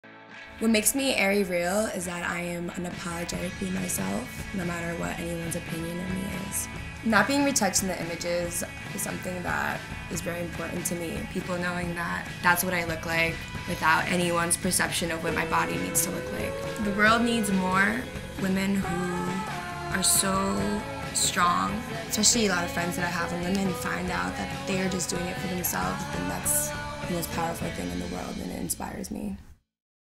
Real Talk With Barbie
No matter what, Barbie is unapologetically herself. Get to know one of the newest faces of AerieREAL!